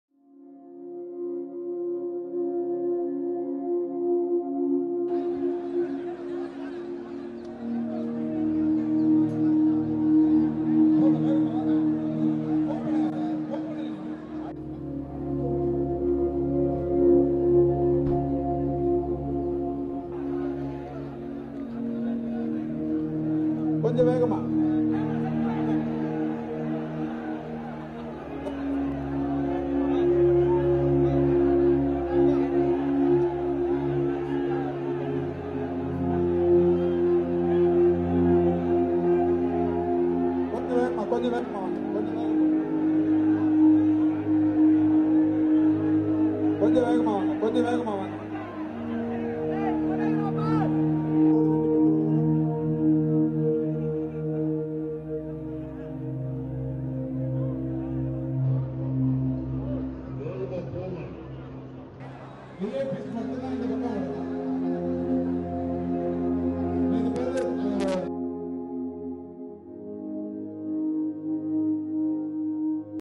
Tamil Nadu CM & Dy CM pay last respects to Karunanidhi at Rajaji Hall - Tamil Nadu
Chennai (TN), Aug 08 (ANI): Tamil Nadu Chief Minister E Palaniswami and Deputy Chief Minister O Panneerselvam reached Rajaji Hall to pay their last respects to DMK chief Karunanidhi. Huge crowd gathered at the Rajaji Hall to see their deceased thalaivar for the last time. DMK chief’s coffin was wrapped in a tricolour.